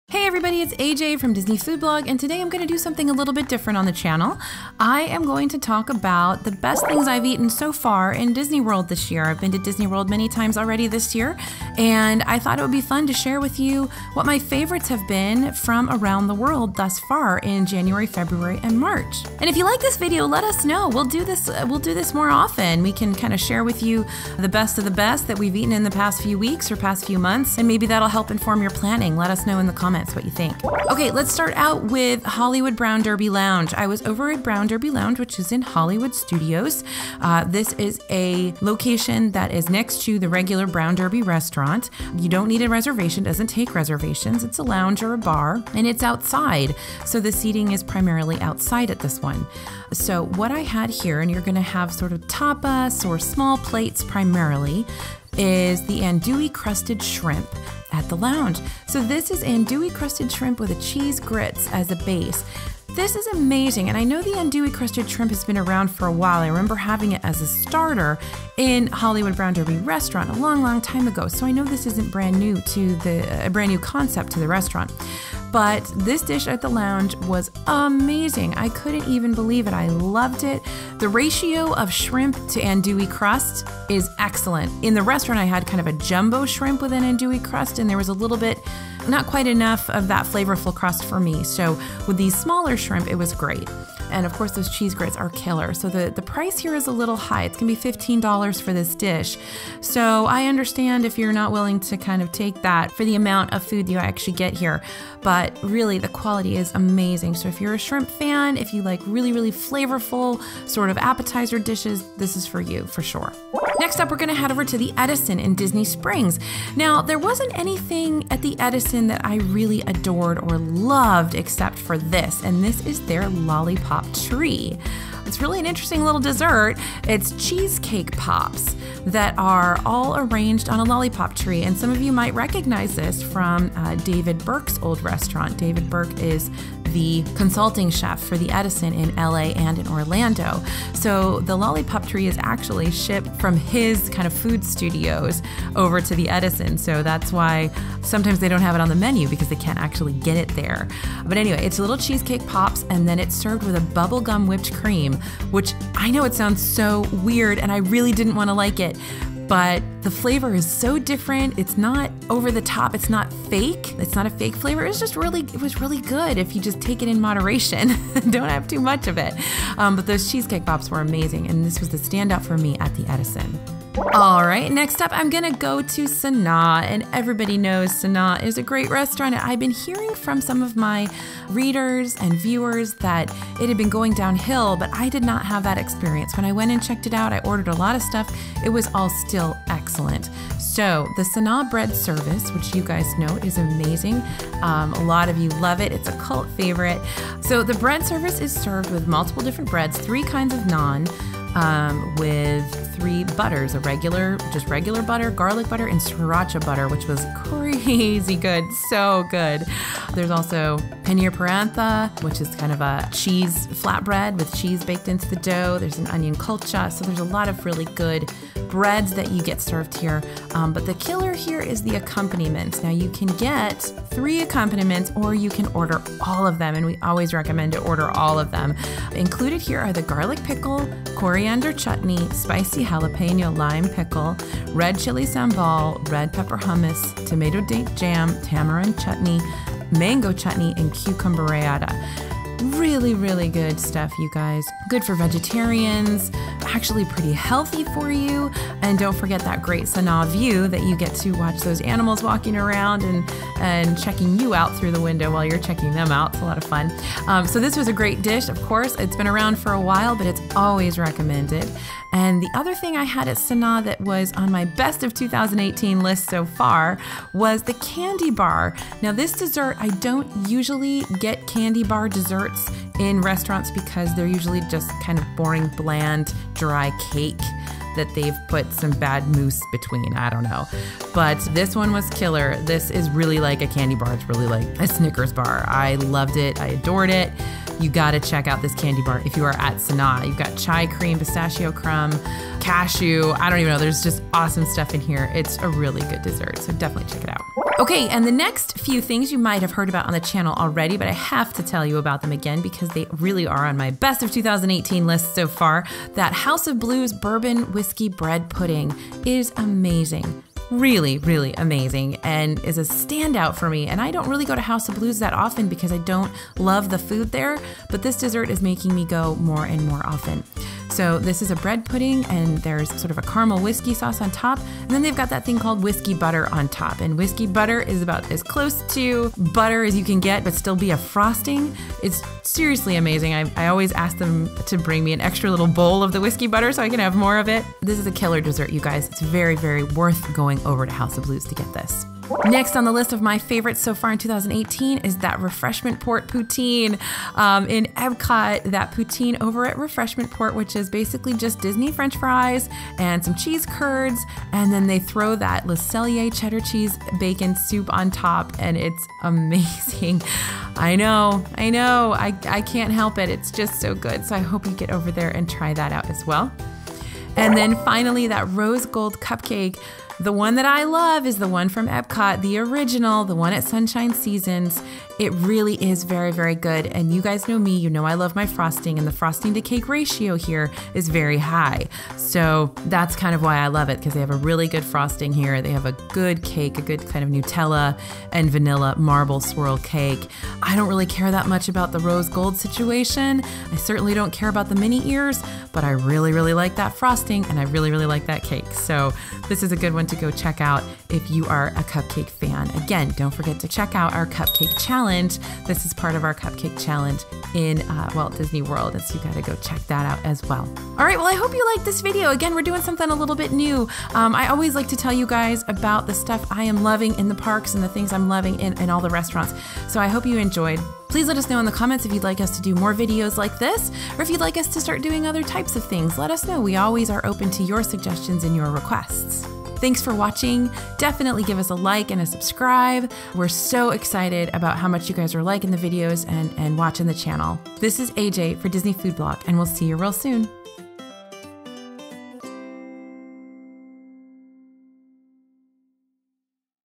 BEST Things I've Eaten IN Disney World in 2018!
Trying something new!! I've had a LOT of new, amazing eats in Disney World in the first three months of 2018, and I wanted to tell you about some of the highlights!